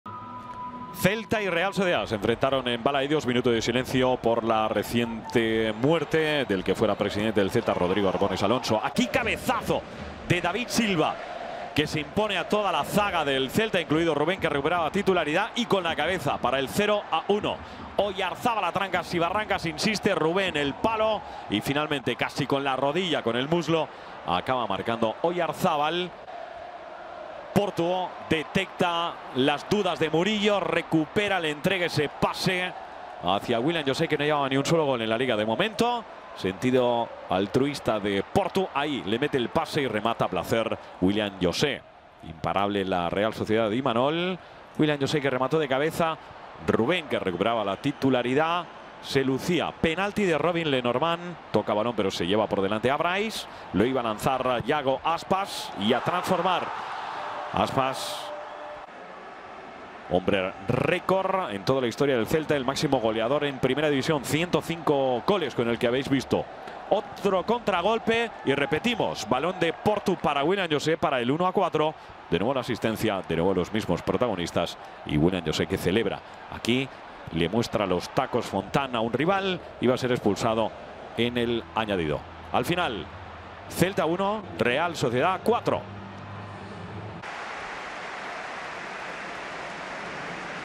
Resumen de RC Celta vs Real Sociedad (1-4)
La Real Sociedad golea en Balaídos con el doblete de Willian José, el estreno goleador de David Silva y el tanto de Oyarzabal. El RC Celta encaja su cuarta derrota en los últimos cinco partidos CeltaRealSociedad J08 LaLiga Santander 2020/2021